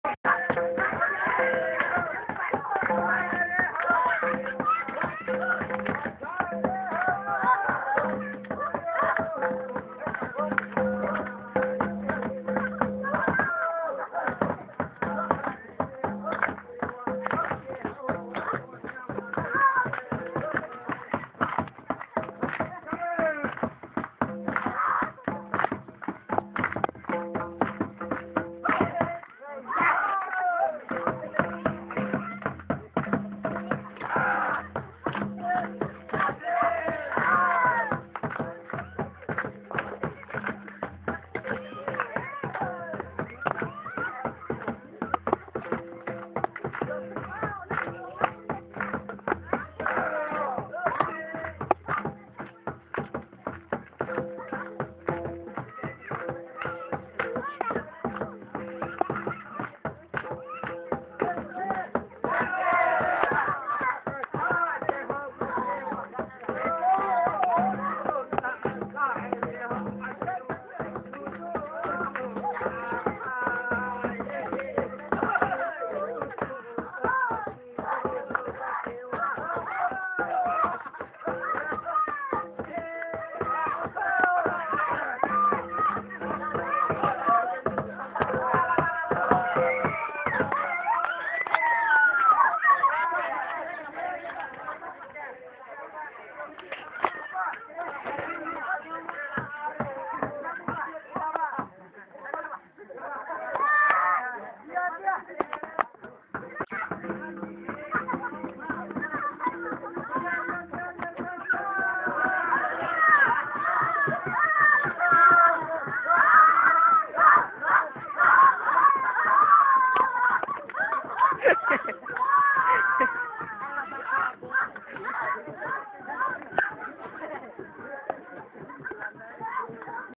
Funny Santhalli dance
Funny Santhali dance.This guys are dancing with out care & there dance steps are awesome.